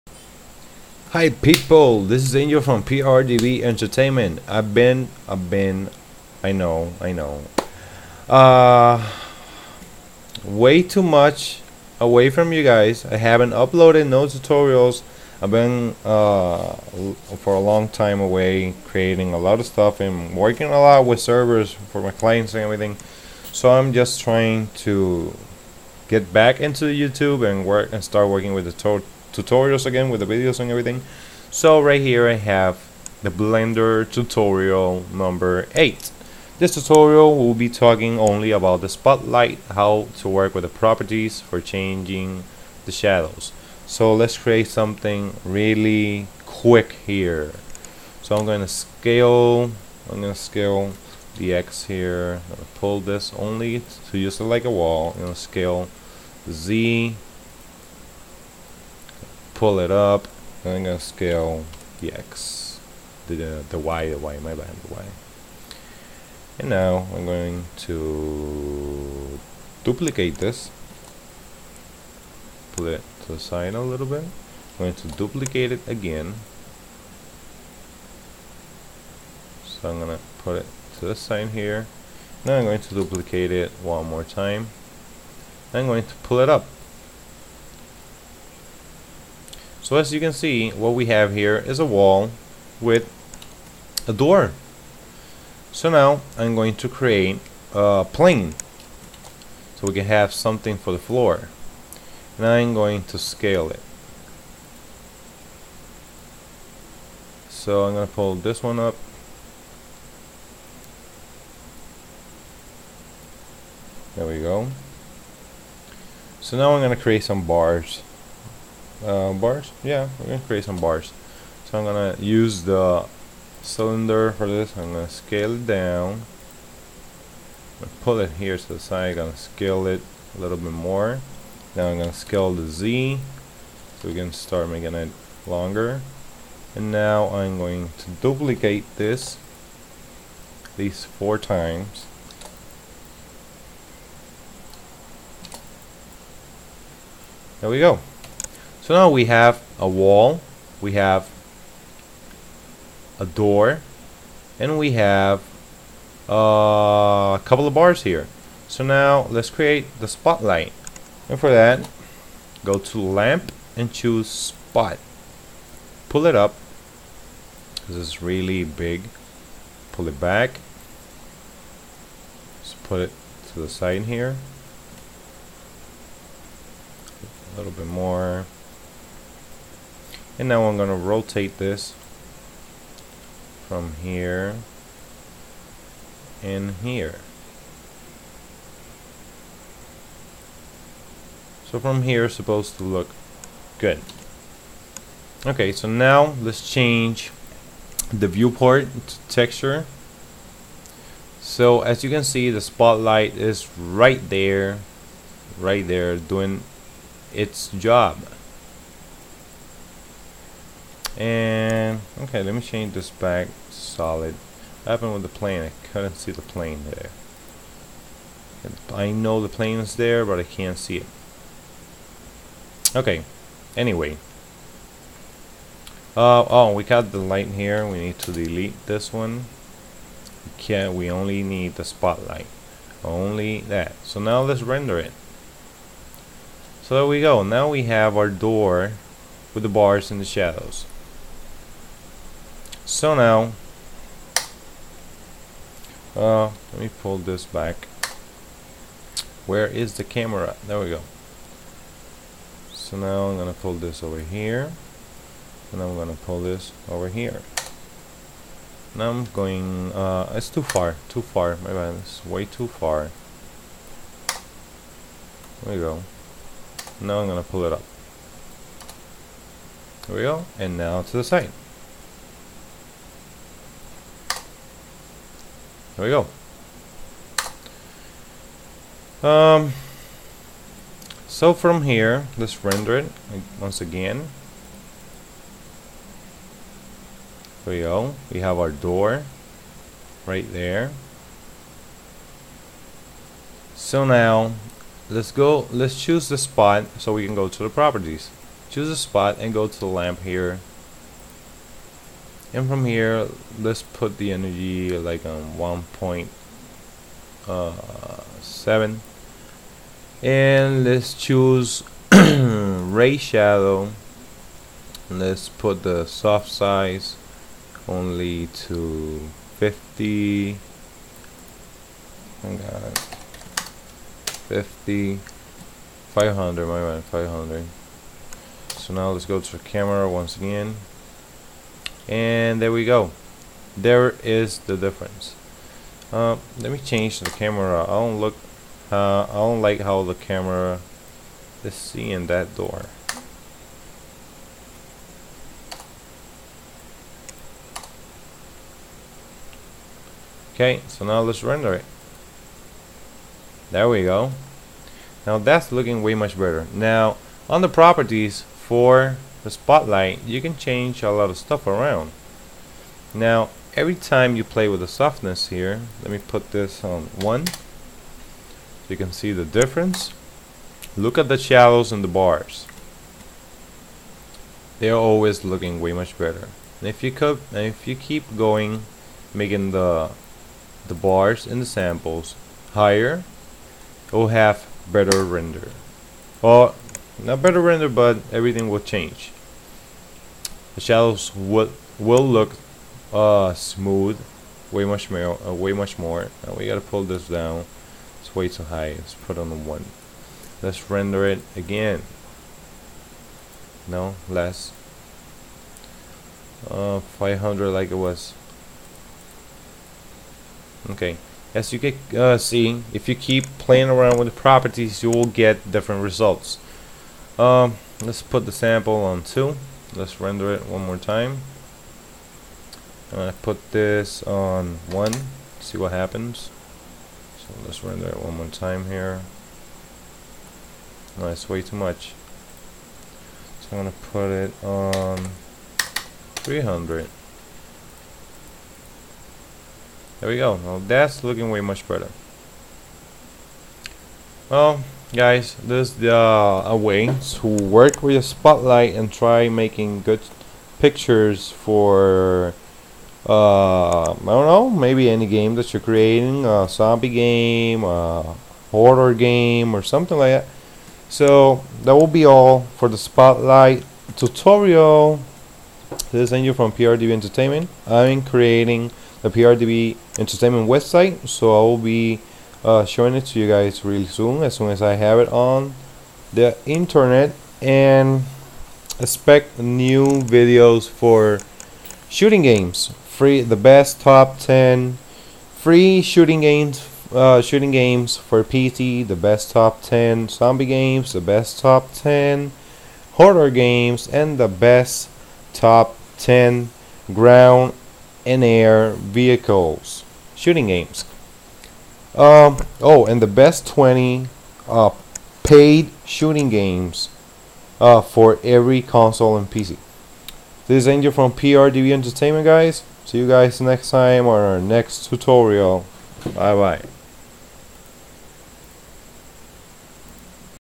Blender 3D Tutorials - Tutorial 8 - The Spot Light
Working a little bit with the best lamp in blender named the SPOT light. Easy stuff, nothing advanced.